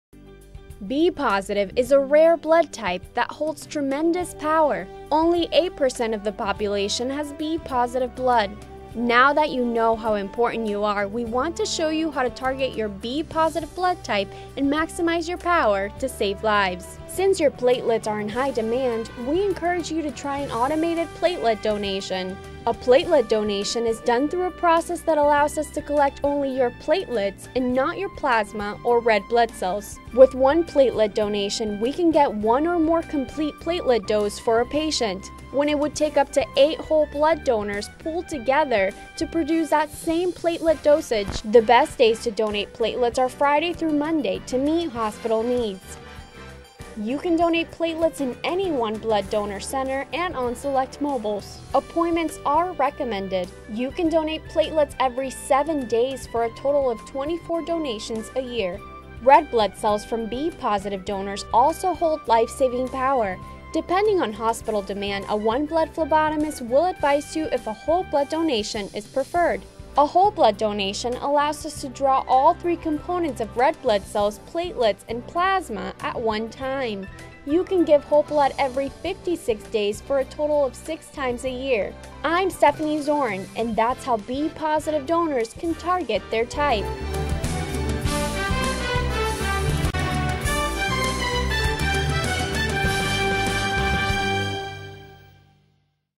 B Positive Blood Type
B+ is a rare blood type that holds tremendous power. Only 8% of the population has B positive blood. B+ blood donors have two ways of targeting the power of their donation.

Learn how to Target Your Type through blood donation on the OneBlood